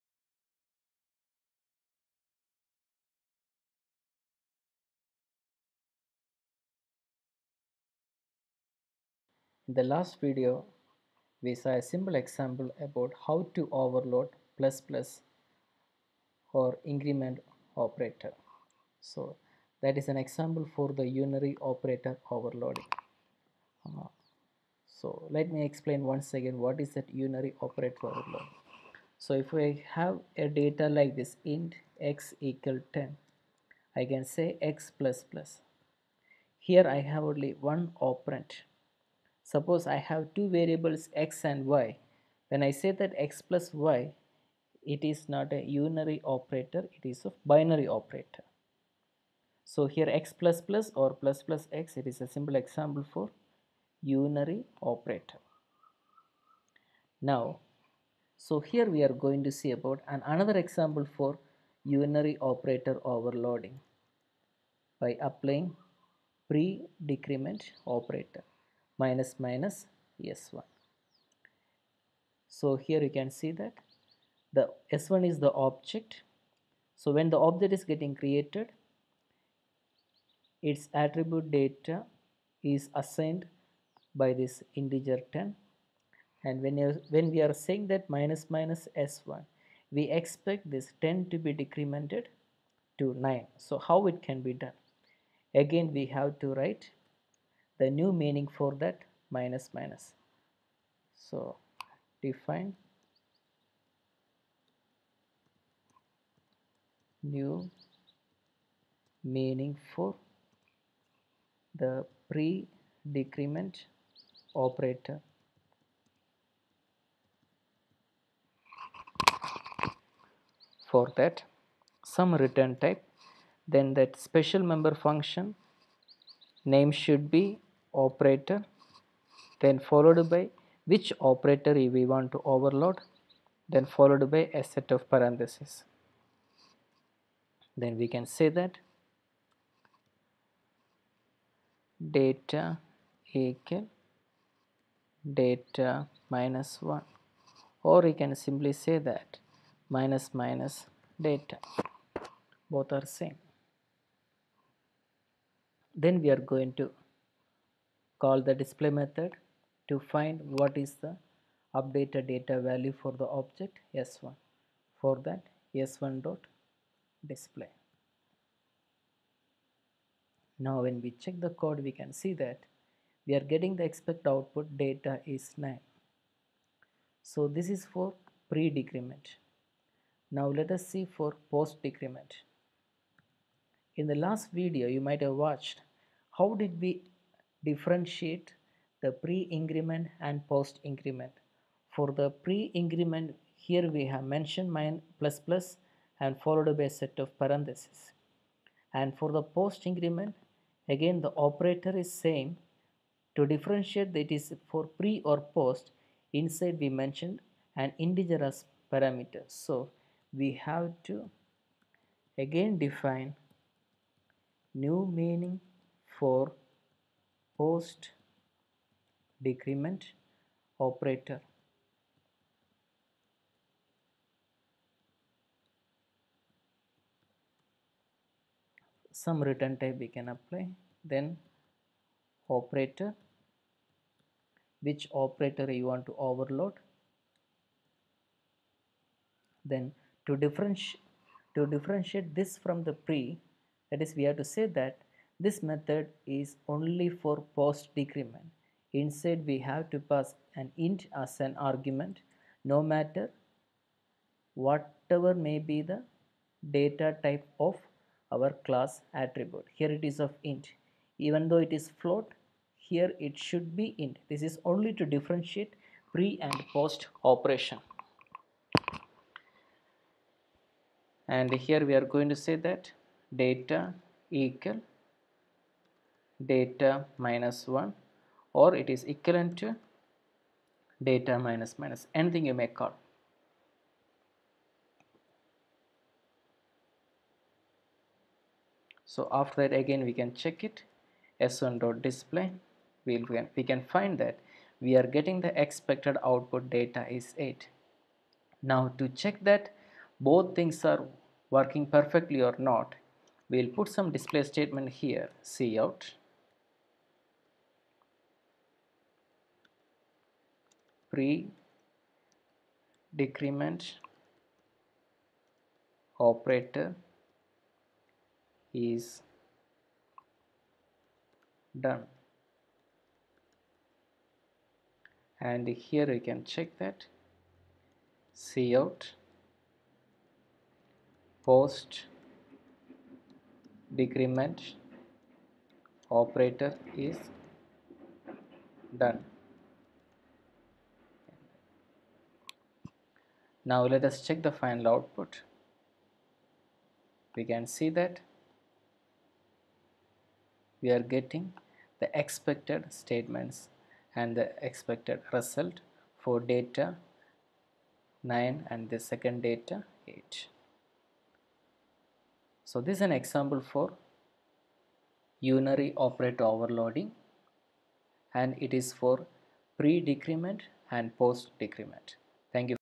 Overload unary(--) operator using C++
This is to demonstrate unary operator overloading for pre decrement and post decrement.